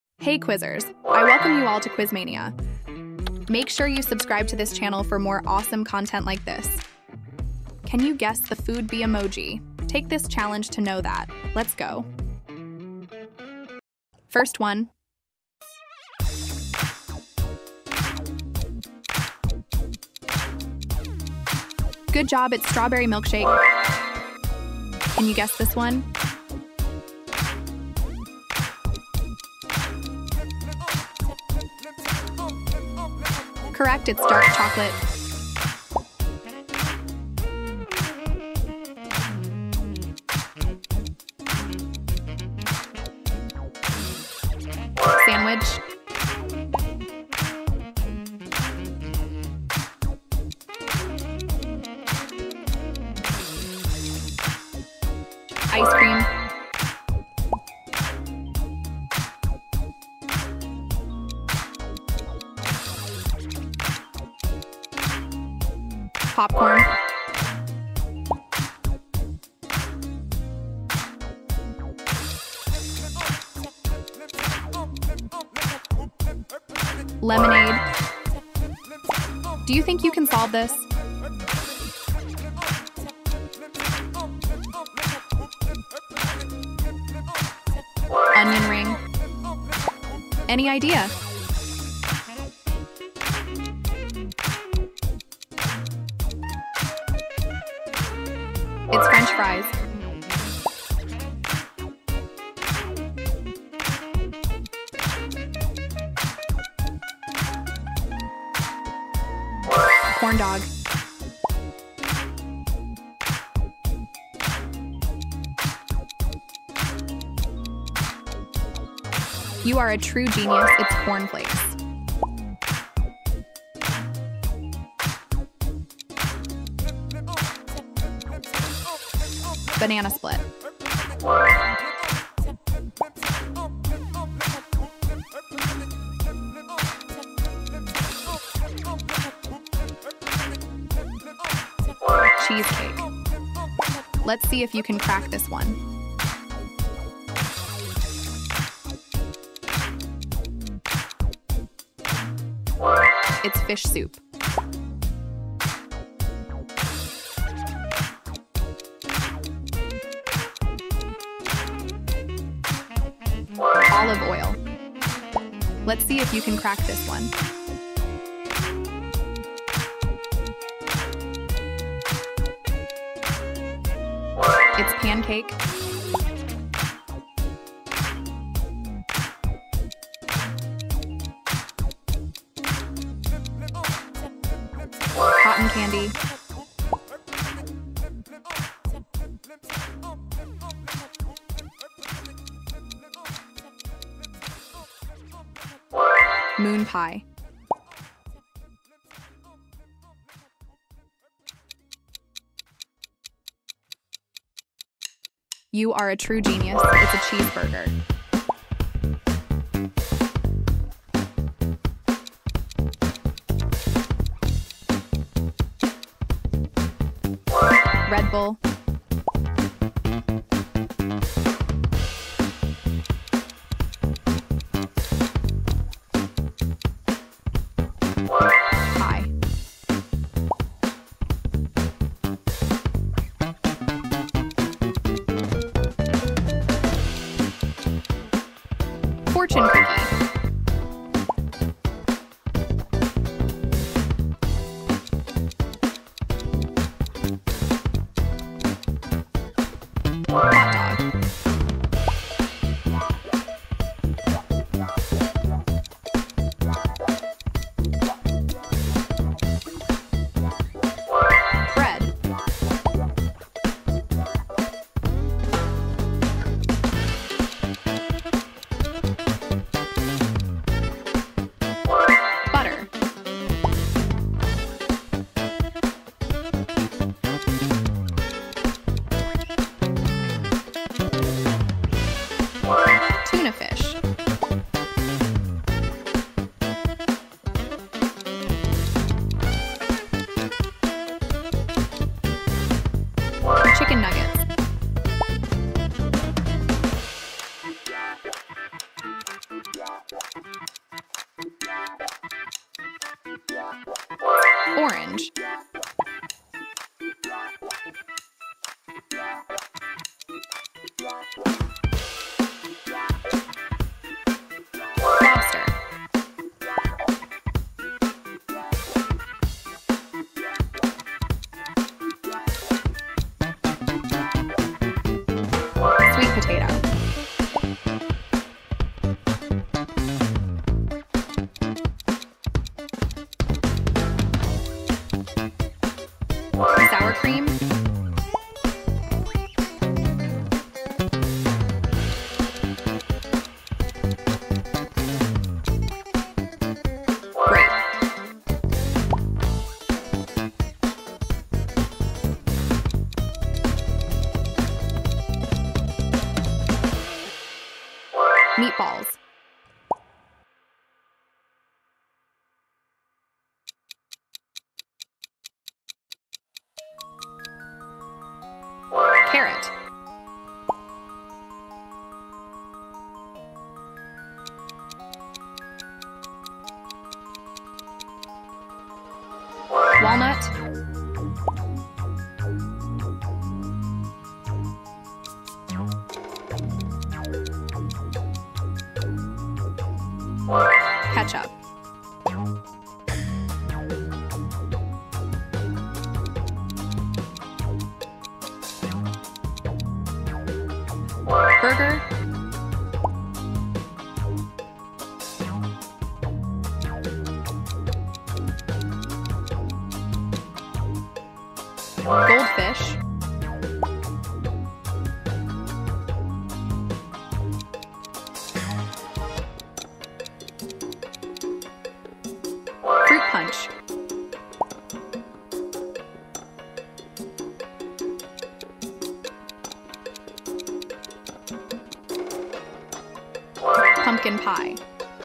Guess The Food By Emoji | Food and Drink by Emoji Quiz | Quiz (Quiz Mania)
This is Guess The Food By Emoji | Food and Drink by Emoji Quiz
Can you Guess The Food by Emoji? This is a fun food and drink quiz you can play with your friends and family! Food game Emoji Challenge

 Guess The Food By Emoji | Food and Drink by Emoji Quiz